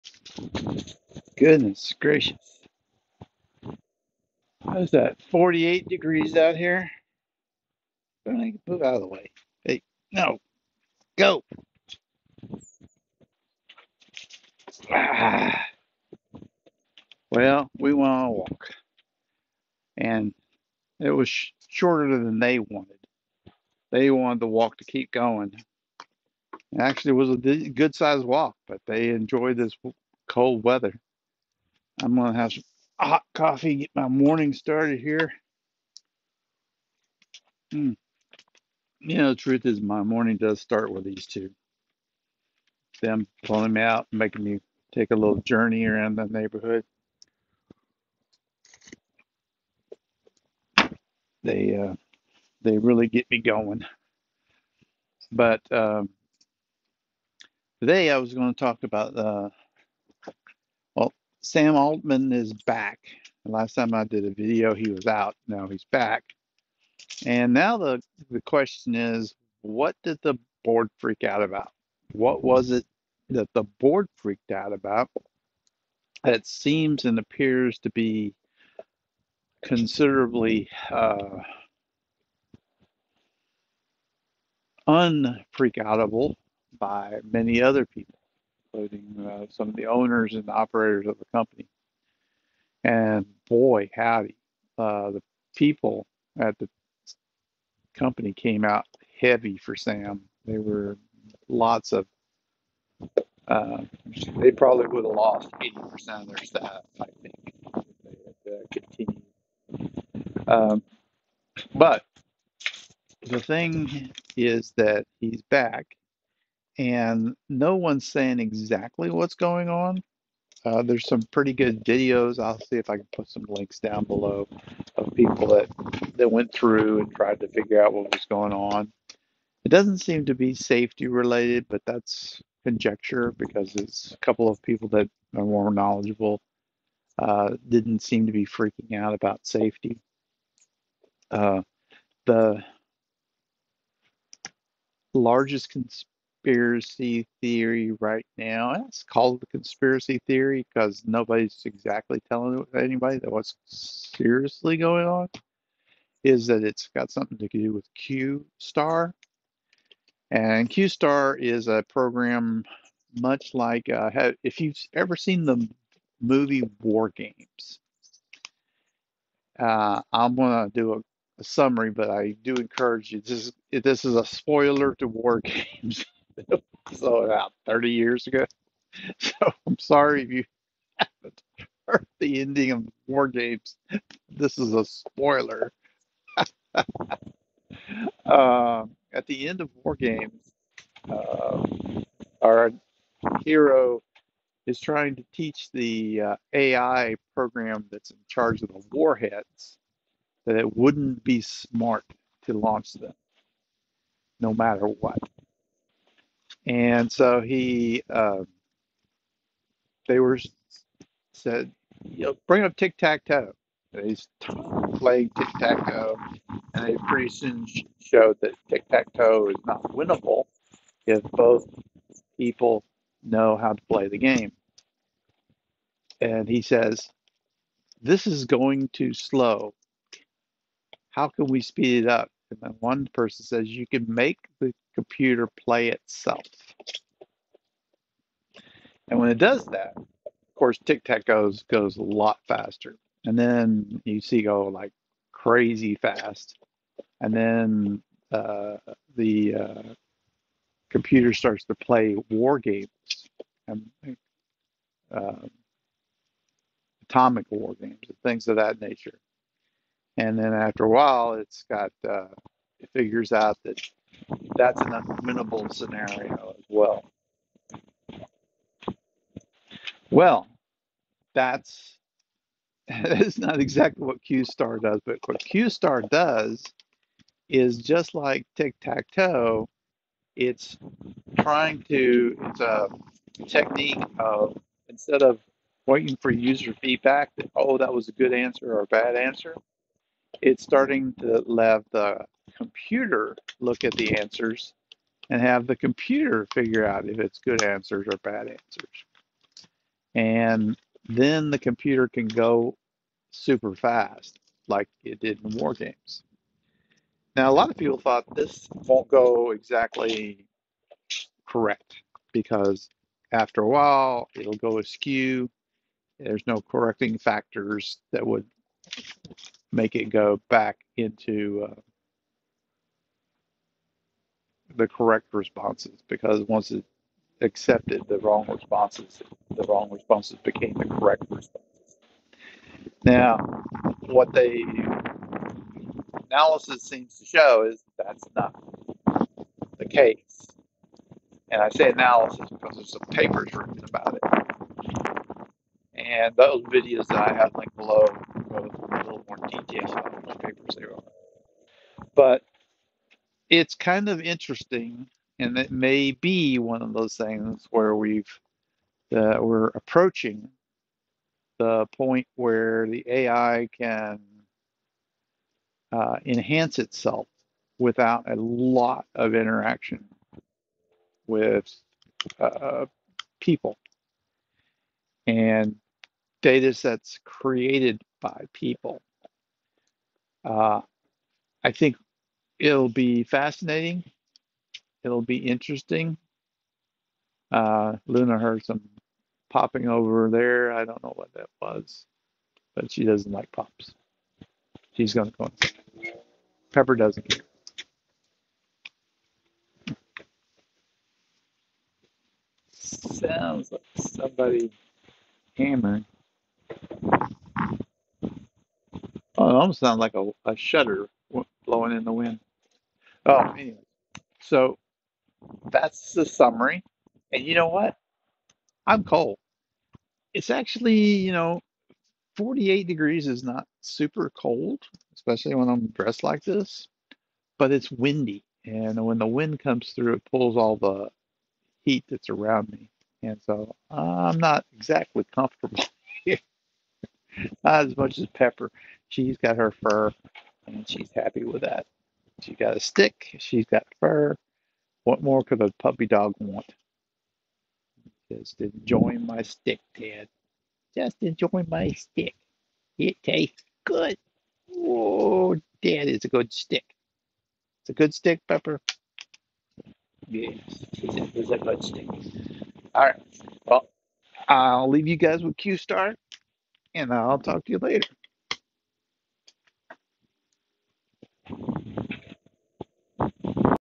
Q*
Well it's a cold day but I want to do a brief update on Open AI's and continuing mystery about why they fired. Sam Altman and one of them is something called q star. so I'm going to briefly talk about what people think it is and why people think it might be the reason he was let go. all this is conjecture. none of its proven. there's not really been a official announcement as to what happened. maybe we'll learn in a few years.